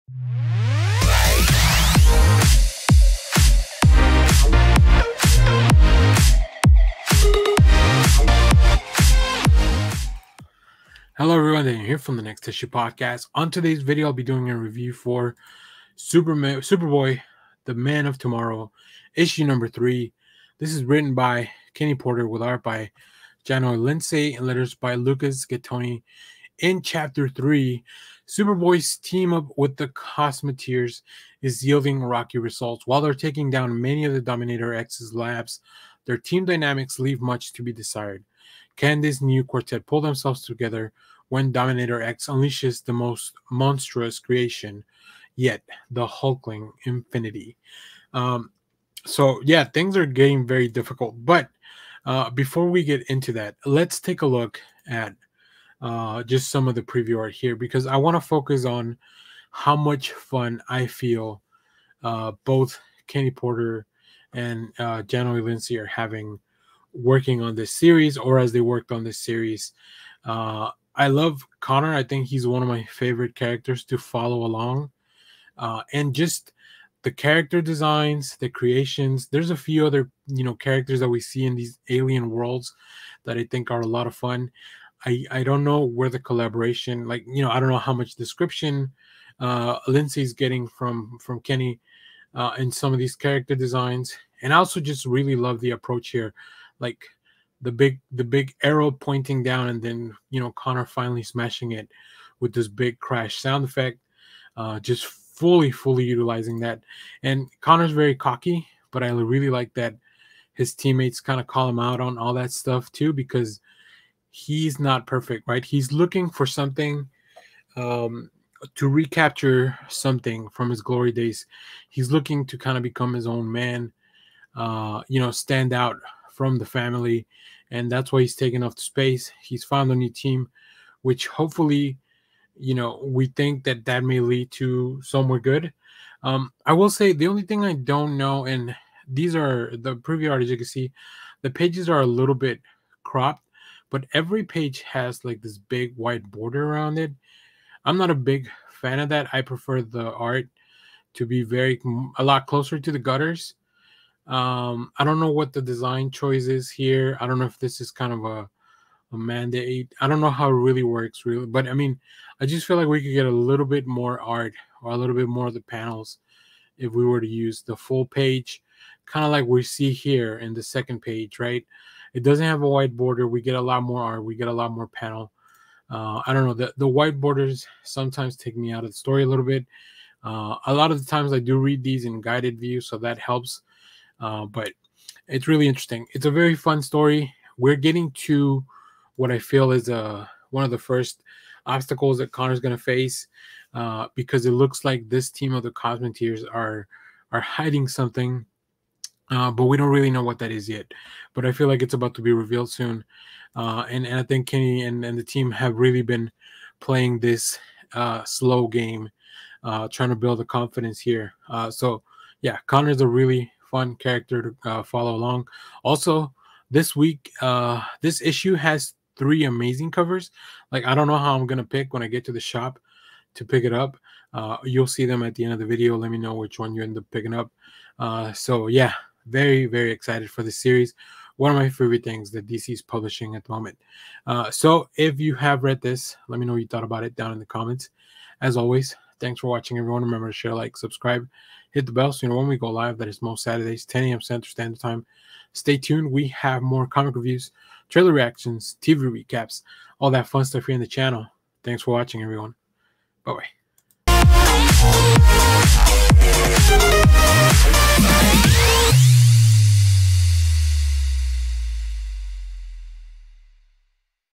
Comic Review | SuperBoy Man of Tomorrow #3 | DC Comics
Written by Kenny Porter Art by Jahnoy Lindsay Letters by Lucas Gattoni

Clay: @Clay_Harrison
Kyle: @Kylepedia
Adrian: @Adrian_Harry
Daniel: @eckospider
Josh: @cosmosis